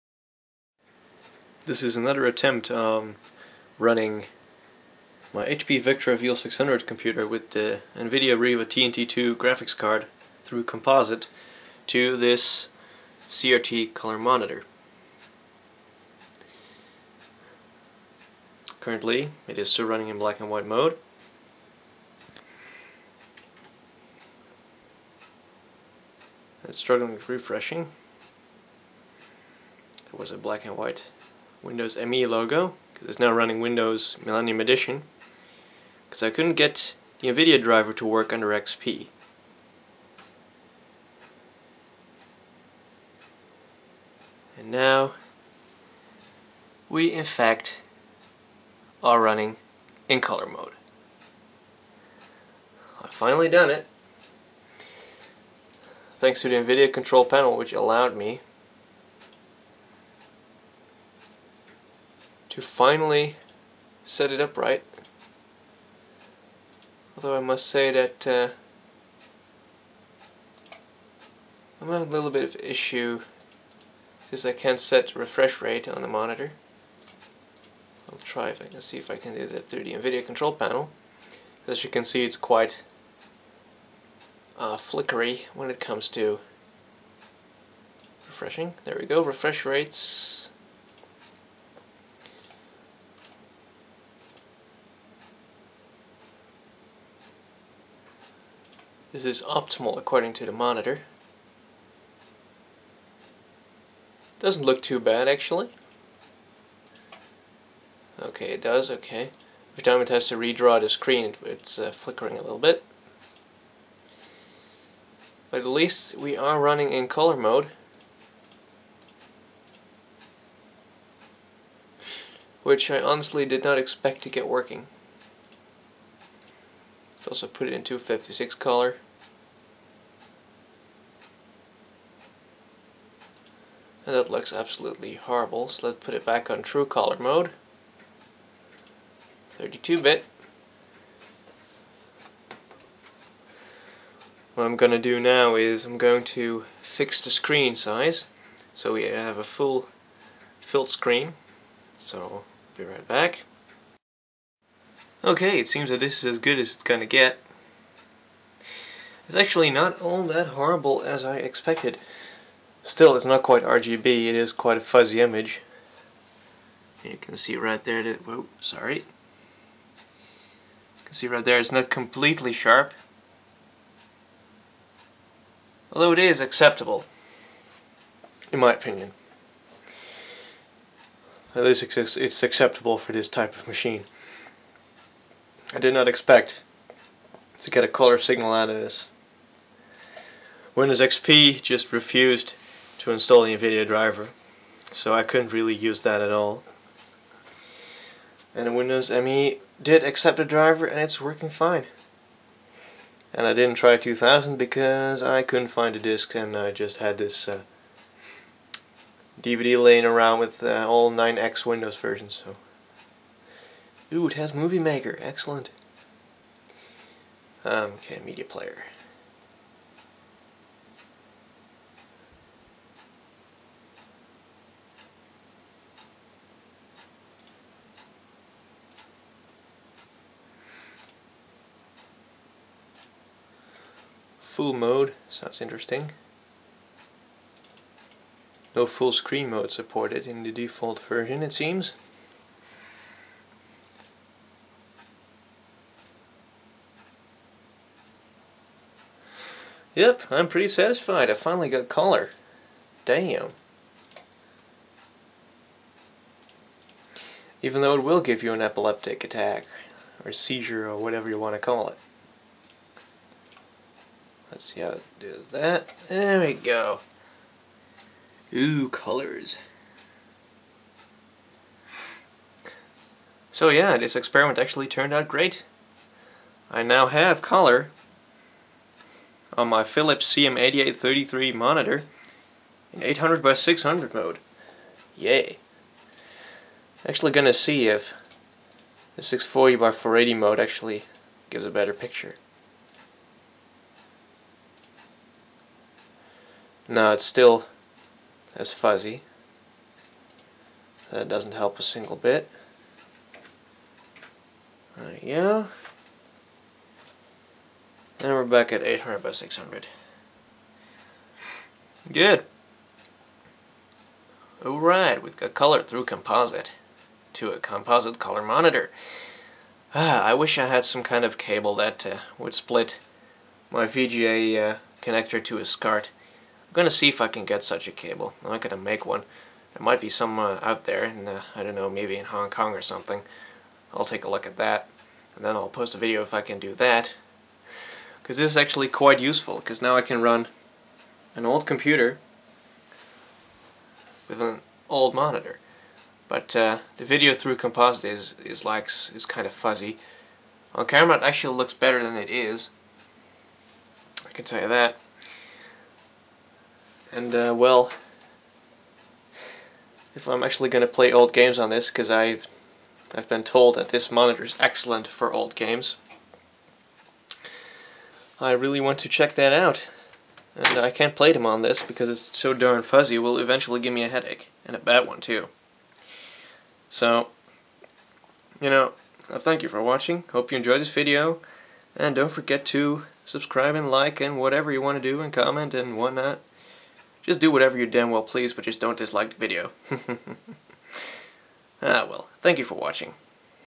HP Vectra VL600 + Color monitor through composite - SUCCESS!
Revisiting a former experiment, succeeding this time. Surprisingly under Windows Mindfuck Edition. (ME)